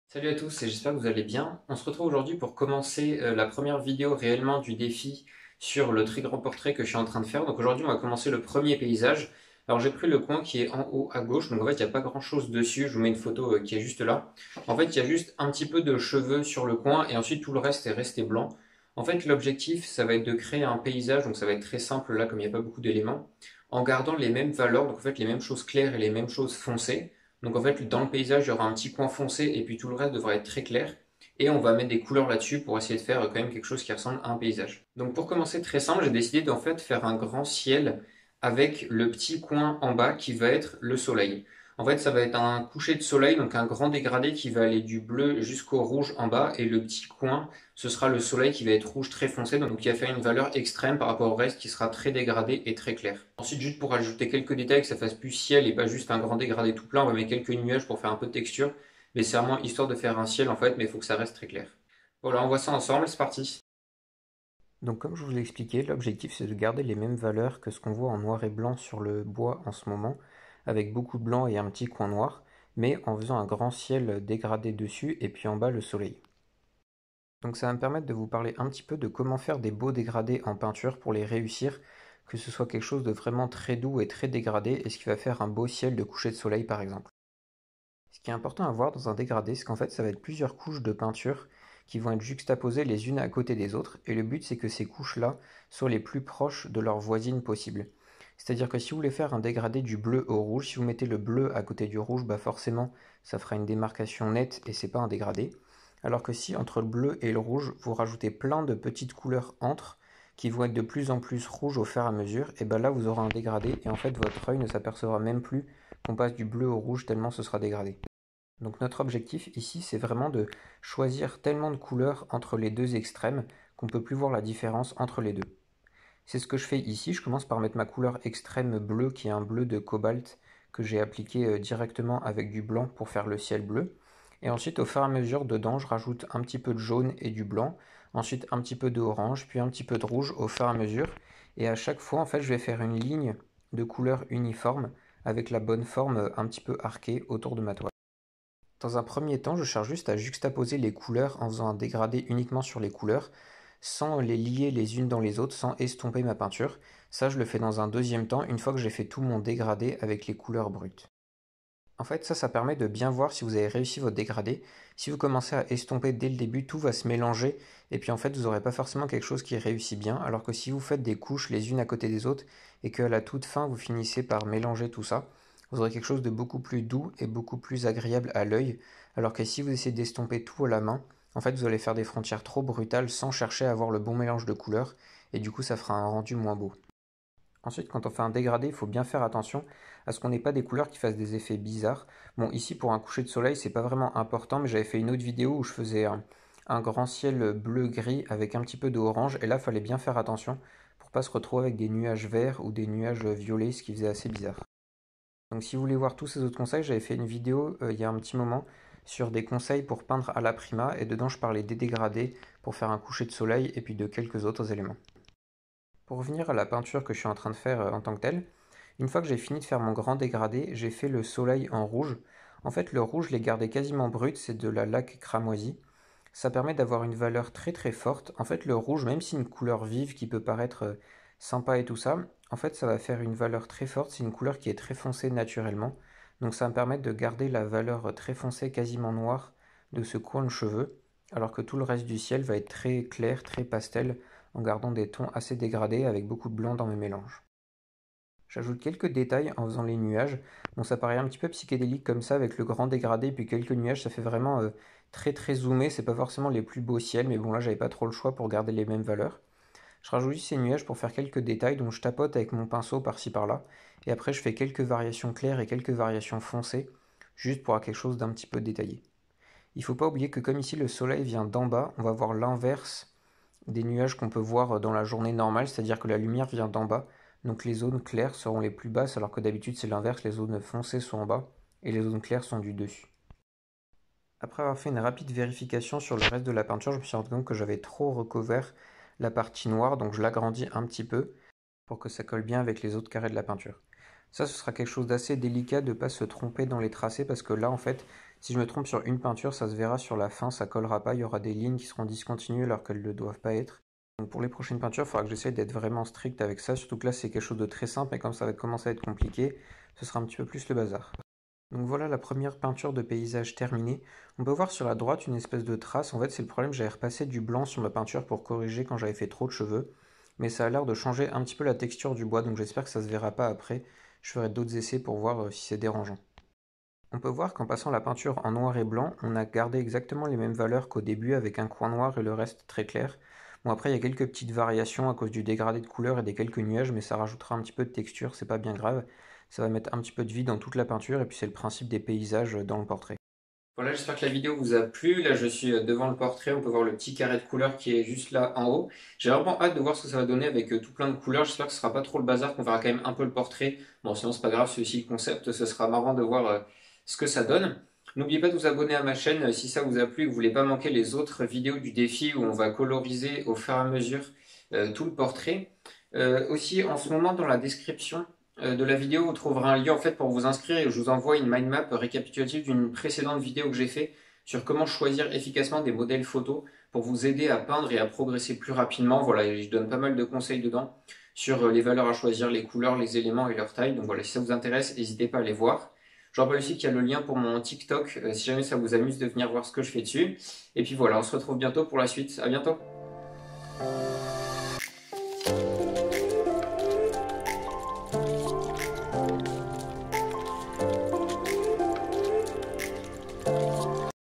Défi peinture mosaïque (1/24) : ciel, nuages et soleil couchant 🎨
Pour la première peinture du défi mosaïque je vais faire un ciel avec quelques nuages et un soleil couchant ! 😉

Pour cette première la peinture est très simpla avec peu de valeurs différentes, mais cela permet de commencer tranquillement !

Pour recevoir la mind map sur le choix de modèle photo en cadeau inscrivez-vous

Pour avoir des infos en avant-première et des exclus dessin et peinture c'est sur TikTok